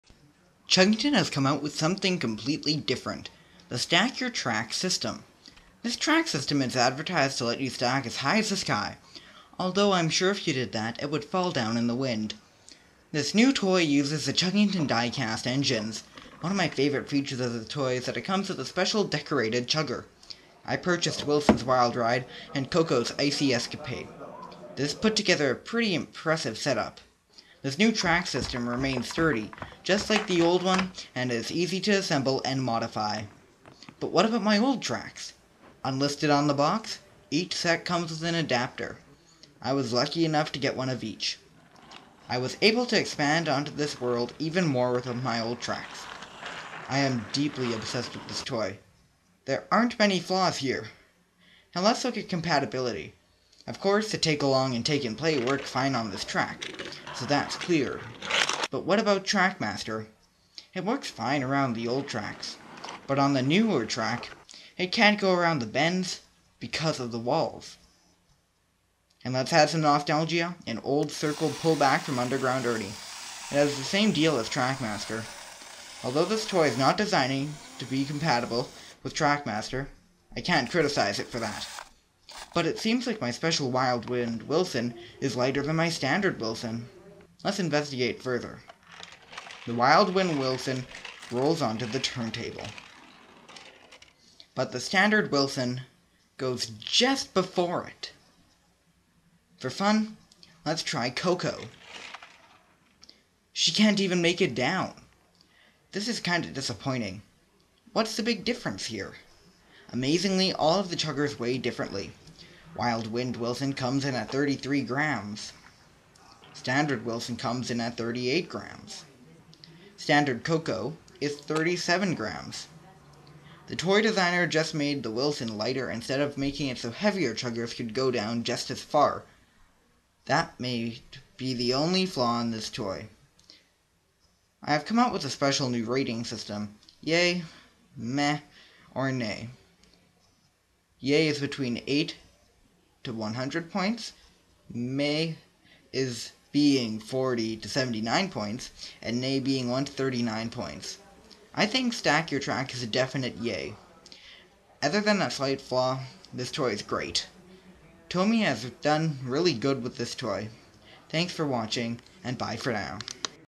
Chuggington StackYourTrack Review
Haven't done a review in a while, but here's a look at the latest in Chuggington Die Cast!